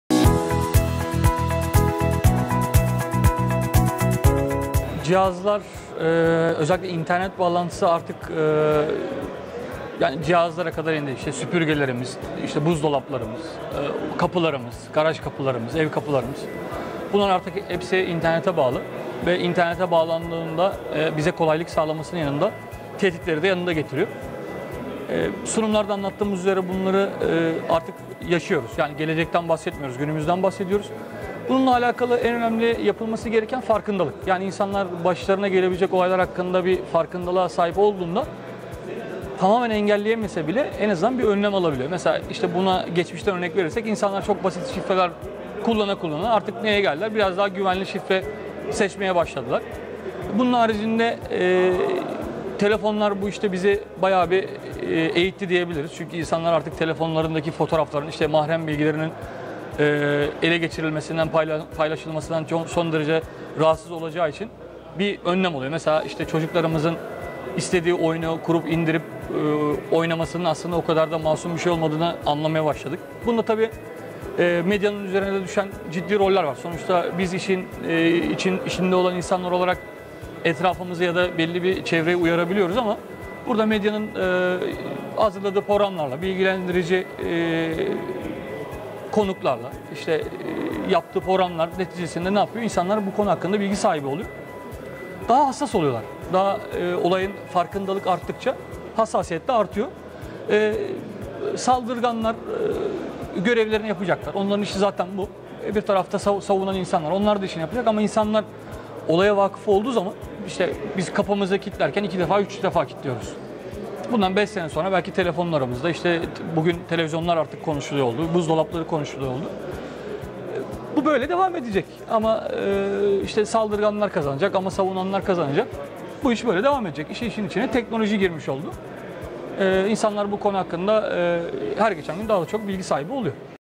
Dünden bugüne siber güvenlik
Siber Güvenlik Uzmanı Kenan Abdullahoğlu siber güvenlik tarihini Wİse TV'ye anlattı.

İnternet bağlantılarının günümüzde ev içi eşyalara kadar geniş bir alanda kullanılmaya başladığını hatırlatan Abdullahoğlu tüm bunların kolaylıklarla birlikte bir çok tehdidi de içerdiğini belirtti.
Kişilerin farkındalık seviyesinin artması gerektiğine dikkat çeken Abdullahoğlu farkındalığın artmasının beraberinde hassasiyetleri de getireceğine değindi.